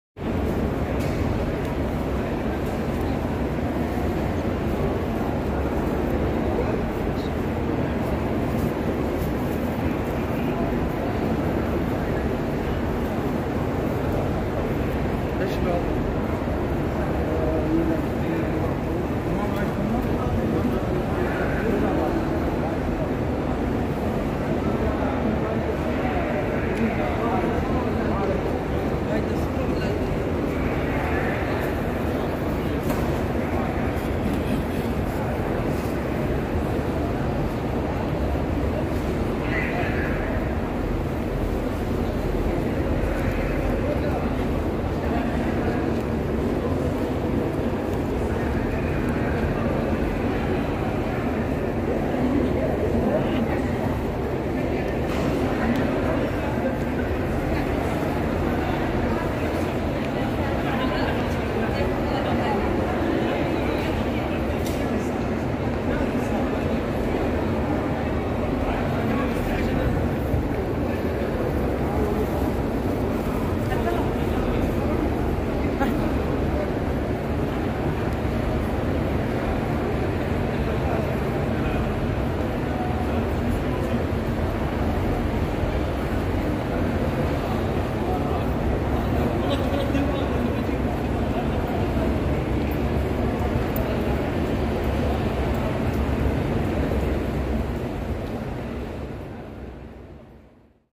Safa and Marowa Makka
It's a part of Haz ,,,,,Hazzi walking this area it's call Safa and Marowa 7 times around this area